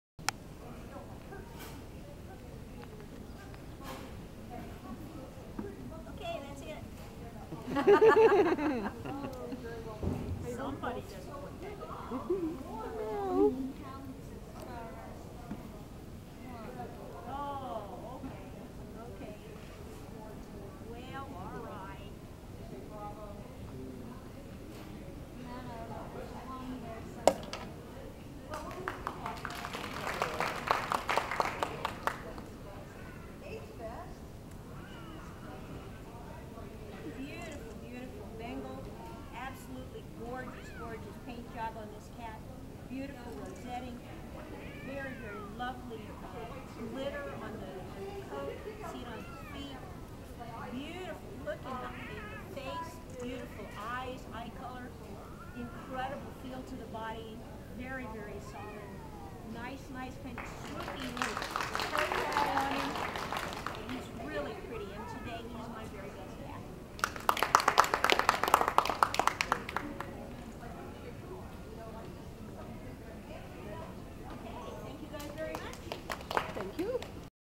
Spotznstripz Prince of chrysoberyl ( Bengal ) first place ,TICA show 2010 , Vickie Fisher
Spotznstripz Prince of chrysoberyl  ( Bengaal ) 11 Mnd.
TICA show 2010  Hellevoetsluis